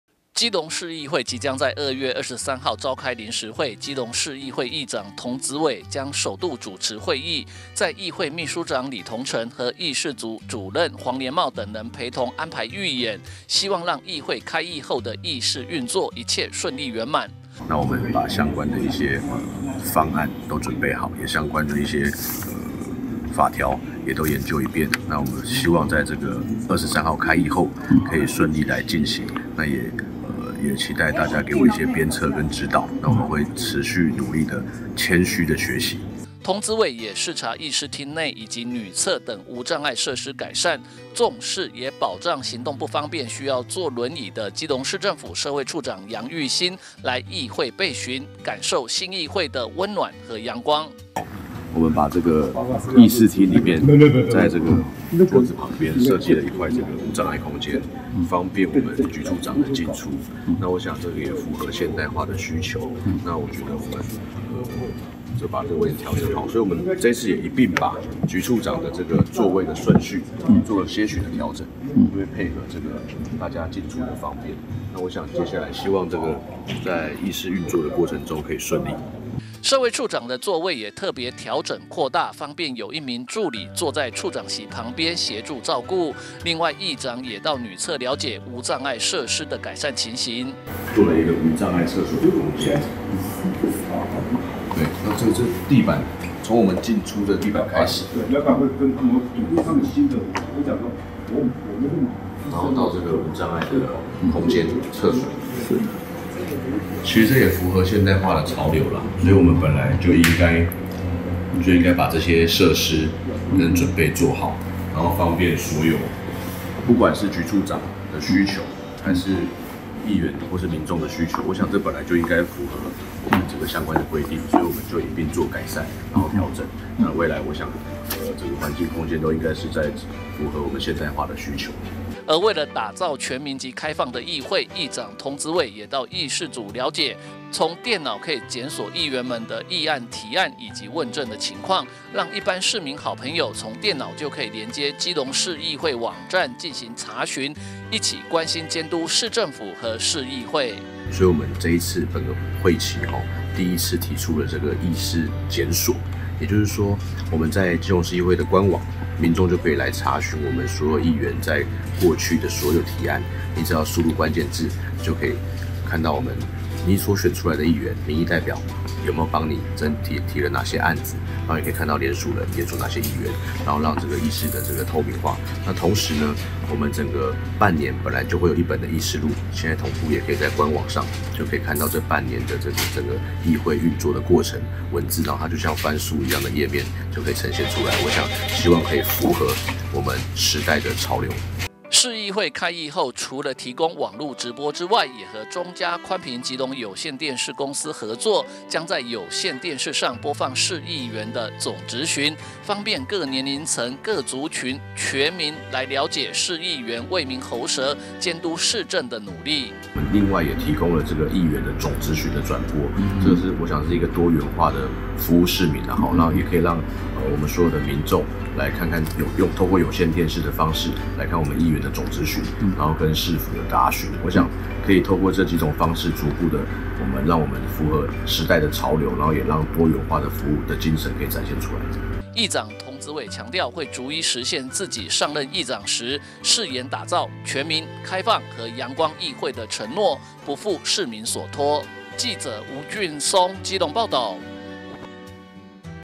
基隆|童子瑋|基隆市議會|謝國樑|楊玉欣|無障礙|議會將開議 童子瑋打造全民開放陽光議會
議會將開議　童子瑋打造全民開放陽光議會

(中嘉吉隆新聞記者吳俊松/基隆報導)基隆市議會即將在２月２３號召開臨時會，基隆市議會議長童子瑋將首度主持會議，因此除了安排預演之外，也視察相關無障礙設施改善以及電腦檢索議員議案和提供有線電視觀看議員質詢等措施，逐一實現自己上任議長時，誓言打造全民､開放和陽光議會的承諾！

基隆市議會即將在２月２３號召開臨時會，基隆市議會議長童子瑋將首度主持會議，在議會祕書長李銅城和議事組主任黃連茂等人陪同安排預演，希望讓議會開議後的議事運作一切順利圓滿！

童子瑋也視察議事廳內以及女廁等無障礙設施改善，重視也保障行動不方便須要坐輪椅的基隆市政府社會處長楊玉欣來議會備詢，感受新議會的溫暖和陽光！

社會處長的座位也特別調整擴大，方便有一名助理坐在處長席旁邊協助照顧，另外議長也到女廁了解無障礙設施的改善！

另外為了打造全民及開放的議會，議長童子瑋也到議事組了解從電腦可以檢索議員們的議案提案及問政的情況，讓一般市民好朋友從電腦就可以連接基隆市議會網站進行查詢一起關心監督市政府和市議會！

而市議會開議後除了提供網路直播之外，也和中嘉寬頻吉隆有線電視合作將在有線電視上播放市議員的總質詢，方便各年齡層各族群全民了解市議員為民喉舌､監督市政的努力！

議長童子瑋強調會逐一實現自己上任議長時，誓言打造全民､開放和陽光議會的承諾，不負市民所託！